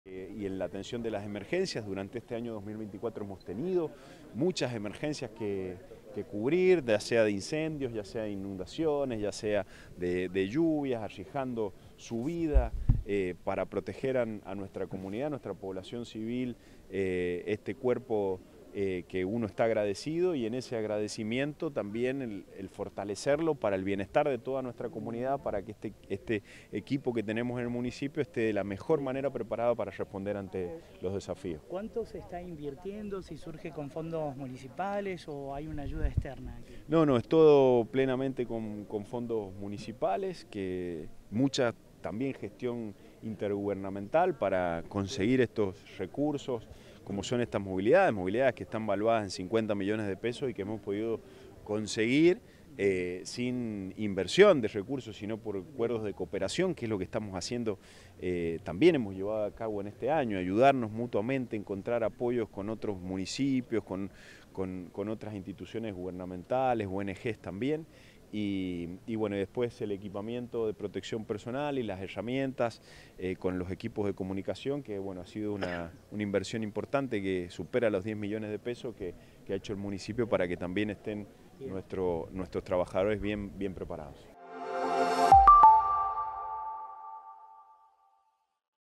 Rawson refuerza su capacidad de respuesta ante emergencias con nuevas movilidades y equipamiento
El intendente Carlos Munisaga anunció la incorporación de una Volkswagen Amarok y herramientas especializadas para Protección Civil y la Dirección de Monitoreo. Con una inversión superior a los 10 millones de pesos y gestiones intergubernamentales que sumaron recursos valuados en 50 millones, el municipio apuesta a fortalecer su capacidad operativa frente a incendios, inundaciones y otros desafíos.